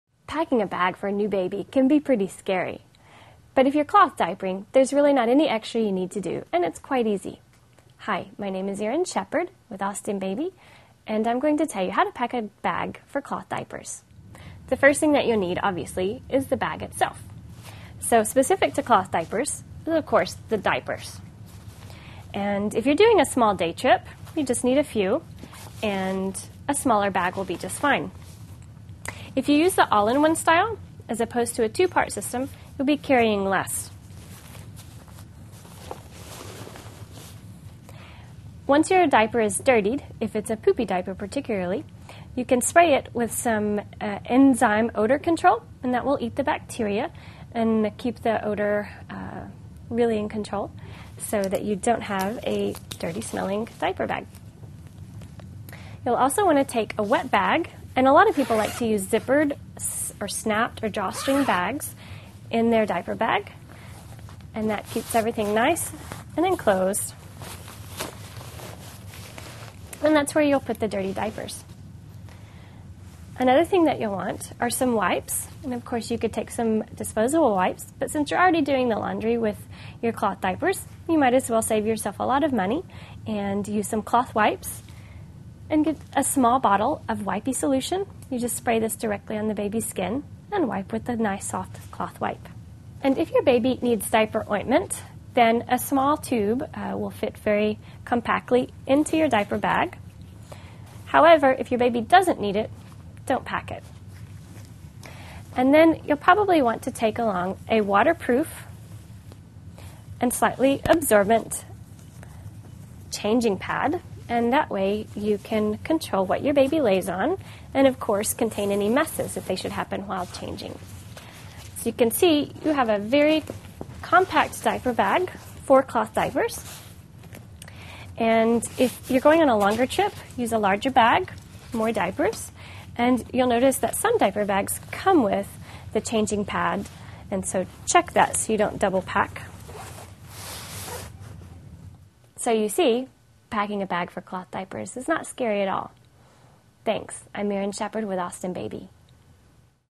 How to Pack a Diaper Bag for Cloth Diapers
How to Pack a Diaper Bag for Cloth Diapers. Part of the series: How to Use Cloth Diapers. Packing a diaper bag for cloth diapers includes packing the cloth diapers, a wet bag, zippered or drawstring bags and wipes. Pack a diaper bag using the cloth diaper tips in this free video.